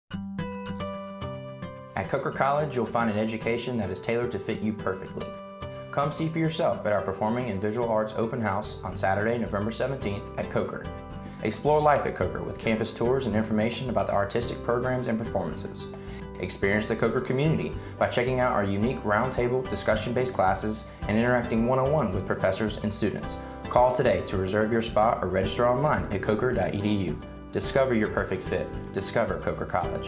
Coker College: Performing and Visual Arts Open House - November 17th, 2012
Coker College will host its Performing and Visual Arts Day on Saturday, November 17th.

Prospective students interested in the arts (along with their families) are invited to experience a glimpse of life as a Coker arts student. In addition to a campus tour and information about financial aid, admissions, and student life, participants will have the opportunity to:
             *Attend one-on-one workshops with faculty
             *Interact with current performing and visual arts students
             *Set up an audition with a professor for possible scholarship money
             *Tour through Coker's studios, musical halls, performance theater, and art gallery.

(Special thanks to Video Production Interns Bill Davis and Jenny Sanborn for their part in the making of this video)